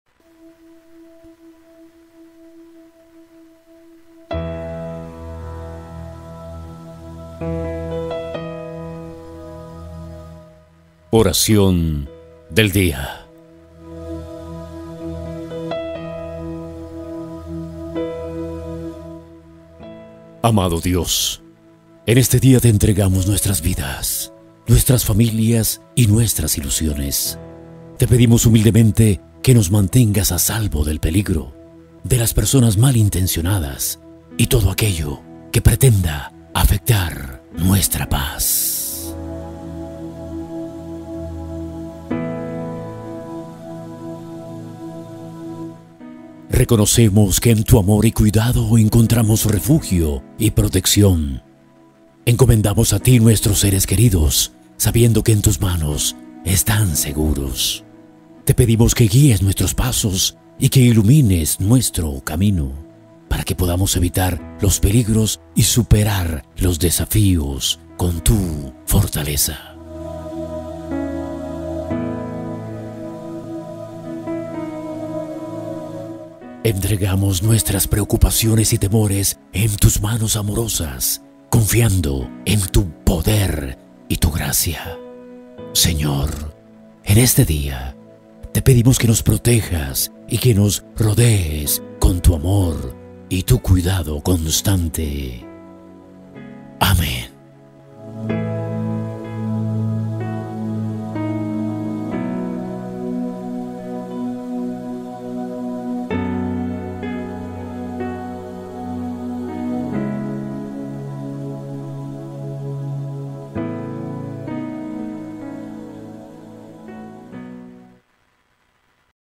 💖ORACION DEL DIA - PARA HABLAR CON PAPITO DIOS👨‍👨‍👧😍
Amado Dios,
En este día te entregamos nuestras vidas, nuestras familias y nuestras ilusiones. Te pedimos humildemente que nos mantengas a salvo del peligro, de las personas malintencionadas y de todo aquello que pretenda afectar nuestra paz.